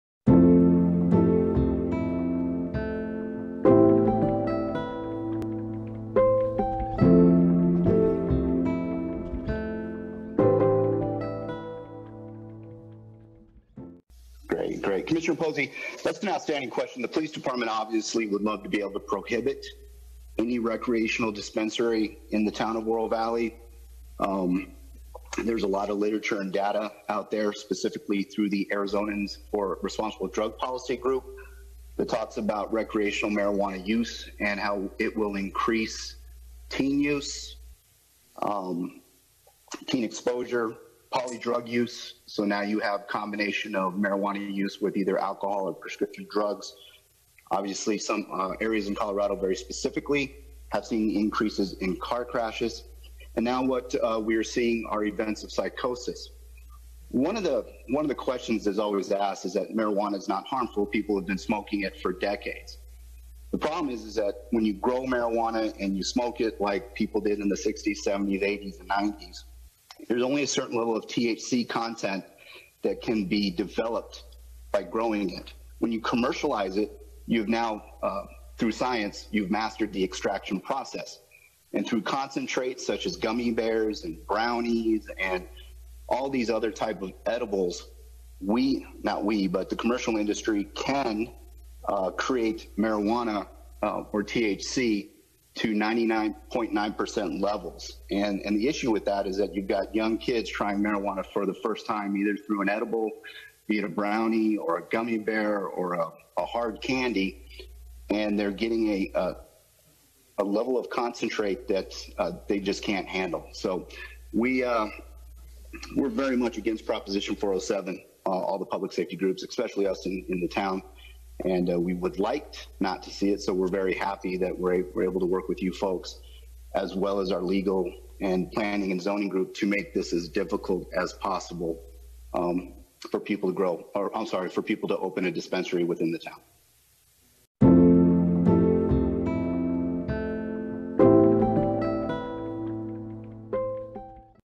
10 27 20 OVPD on Prop 207
Town Staff presented revisions to Town Code to ensure legal requirements are in place if Prop 207 passes (legalization of recreational marijuana).  One of the Commissioners asked OVPD for an opinion of the proposition.